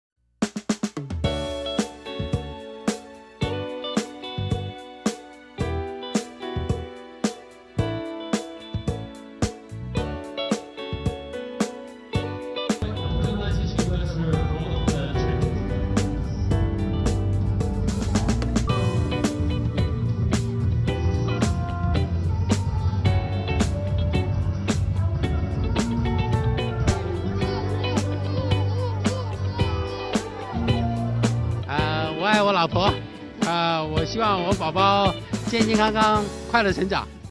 Pedalling for love! People in China's Shanghai celebrate Valentine's Day in style
Many people have flocked to the Oriental Pearl Tower, a landmark in east China's Shanghai, to celebrate Valentine's Day by pedalling. Specially designed stationary bikes have been installed around the tower. As riders pedal, the bikes generate electricity which then helps light up the tower in red. Watch it!